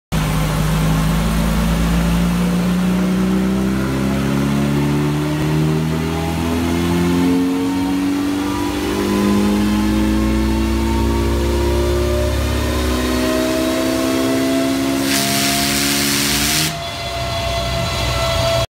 MG TC at Turbopacs for power run and tuning
Lovely MG with supercharger conversation on the rollers at Turbopacs